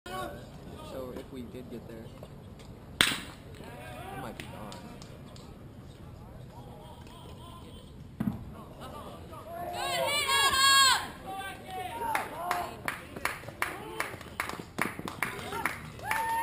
Adam Adkins Freshman Lamar CC 2015
Hits it just shy of a home run off the center field fence. This was with a wood bat.